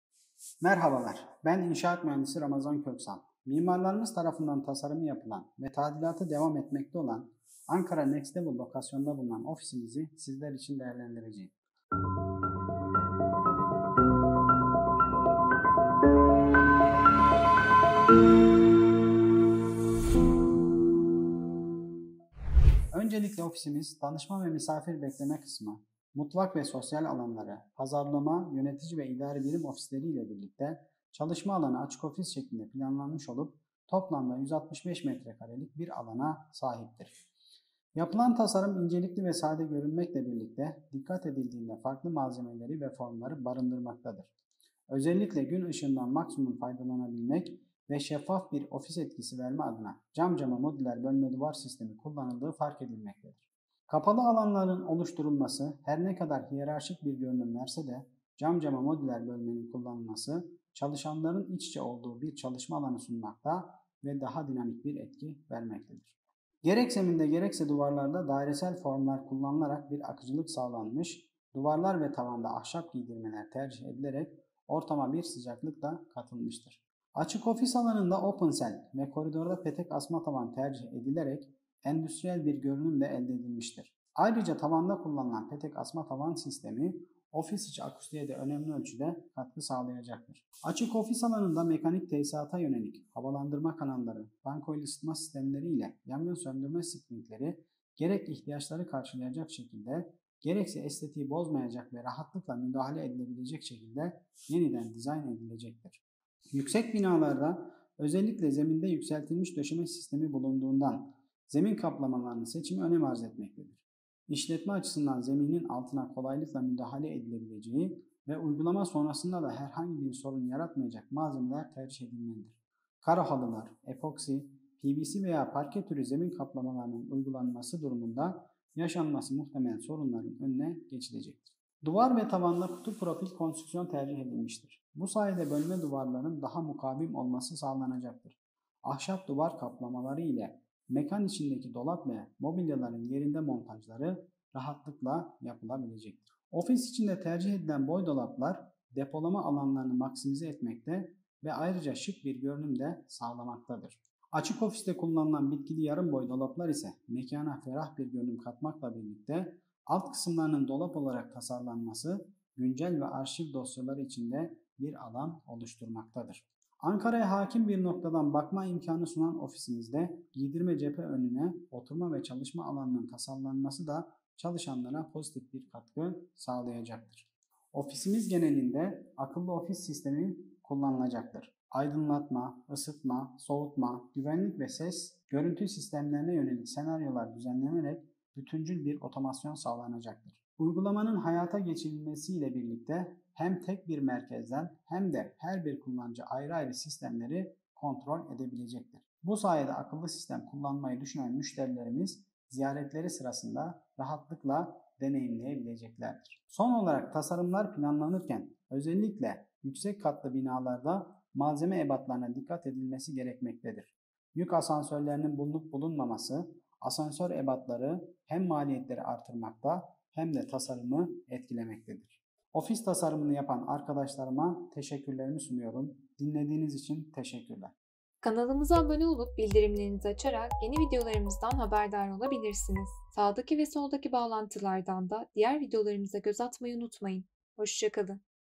FARKLI MALZEMELERİN ORTAK DİLİ | AKILLI OFİS SİSTEMLERİ
İnşaat Mühendisimiz Ramazan Bey, Efeoğlu Proje ekibi tarafından tasarımı yapılan ve Ankara Next Level'da tadilatı devam eden ofisimizi sizler için değerlendirdi.
165 metrekare net alana sahip ofisimiz farklı malzeme ve farklı formları bir arada bulundurmaktadır.
Güneş ışığından maksimum oranda faydalanmak ve hiyerarşiden uzak, şeffaf bir çalışma alanı elde edebilmek için fazlaca cam alanlara yer verilmiştir.
Yer yer kullanılan ahşap malzeme detayları ortama sıcak bir hava katmıştır.
Ayrıca ofisimizin genelinde akıllı ofis sistemi kullanılacaktır. Aydınlatma, ısıtma, soğutma, güvenlik, ses, görüntü sistemlerinde bütüncül bir otomasyon sağlanarak merkezden veya her bir kullanıcı tarafından ayrı ayrı kontrol edilebilecektir.

Akıllı ofis sistemi ve ofis tasarımındaki malzeme detaylarının tamamını videomuzda bulabilirsiniz.